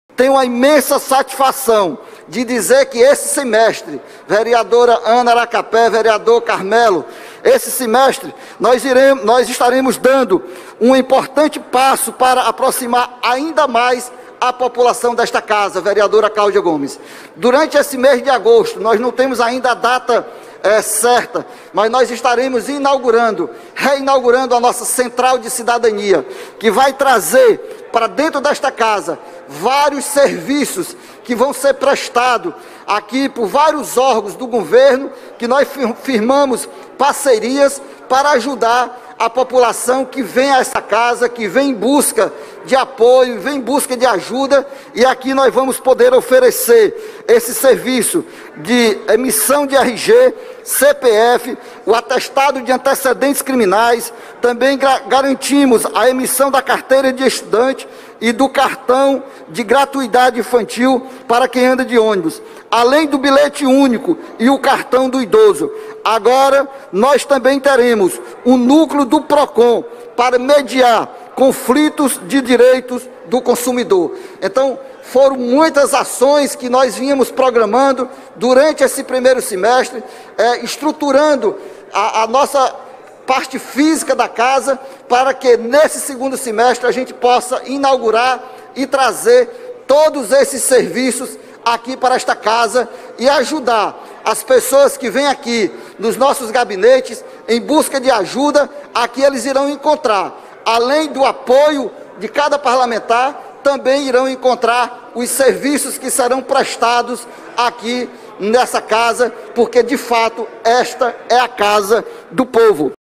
Antônio Henrique comenta sobre projetos à população fortalezense
Os vereadores de Fortaleza retomaram, durante solenidade presencial, na manhã desta segunda-feira (02), os trabalhos do segundo semestre deste ano. O presidente da Câmara Municipal, o vereador Antônio Henrique (PDT), destacou o empenho de seus pares para aprovar matérias de interesse da população, destacando que a Casa Legislativa estará mais próxima da população durante os próximos meses, com a abertura de novos equipamentos.